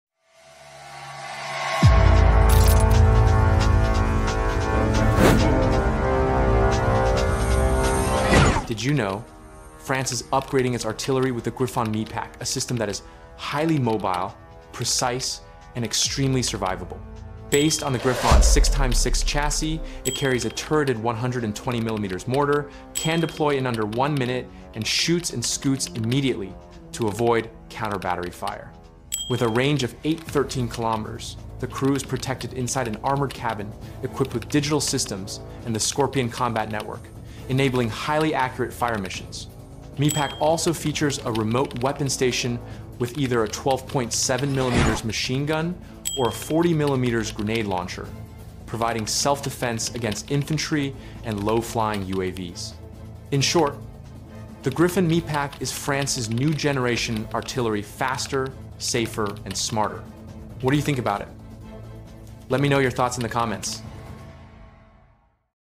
Griffon MEPAC: France’s Next-Gen Artillery – Faster, More Accurate, More Survivable!
The Griffon MEPAC is France’s next-gen artillery system—fast, accurate, and built to survive modern battlefields. A turreted 120mm mortar, rapid “shoot-and-scoot” tactics, digital fire-control, and a remote weapon station make it one of the most advanced artillery platforms today.